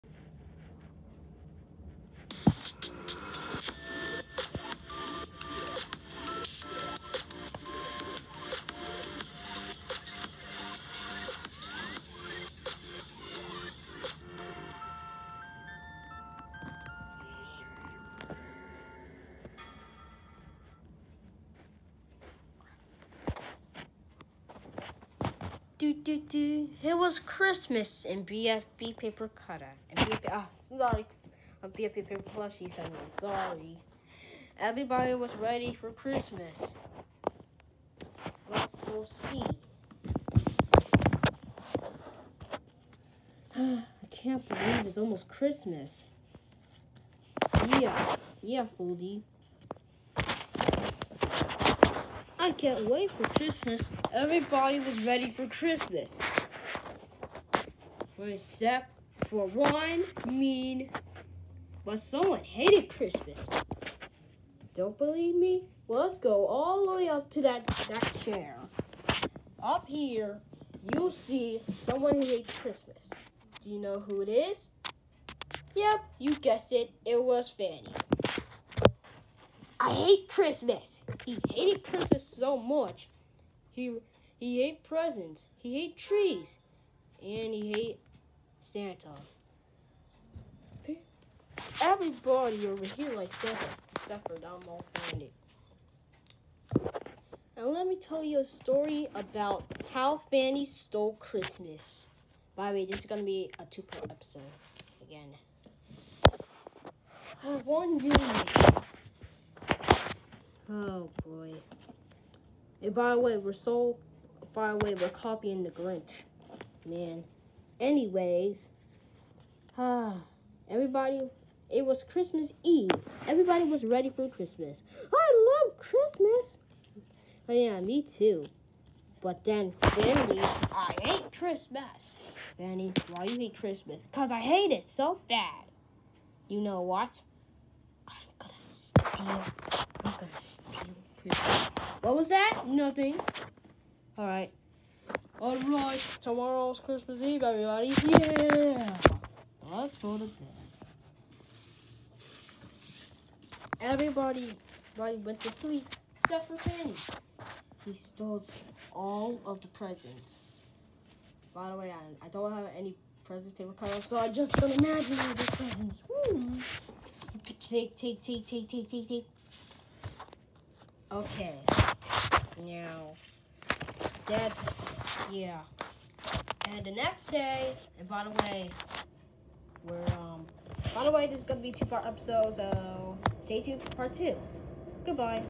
BFB paper plushy‘s episode 5 how fanny stole Christmas part 1
I’m not copying The Grinch I’m just making parody this is a Christmas special